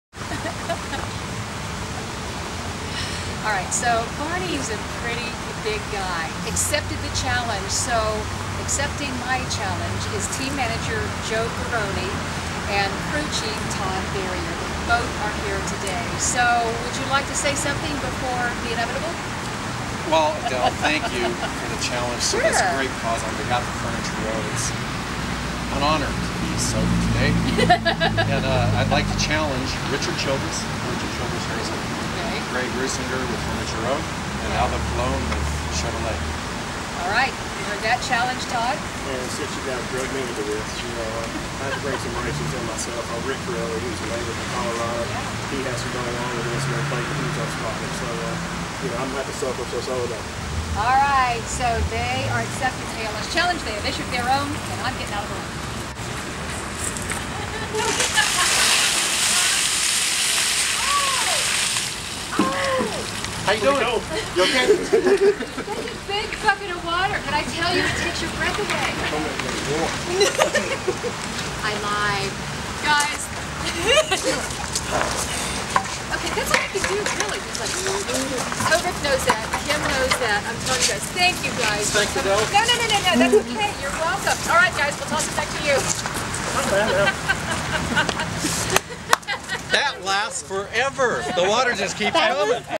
Behind the Scenes - Furniture Row Racing Ice Bucket Challenge
Crew chief Todd Berrier and GM Joe Garone of Furniture Row Racing take on the Ice Bucket Challenge issued by KUSA 9News' Adele Arakawa.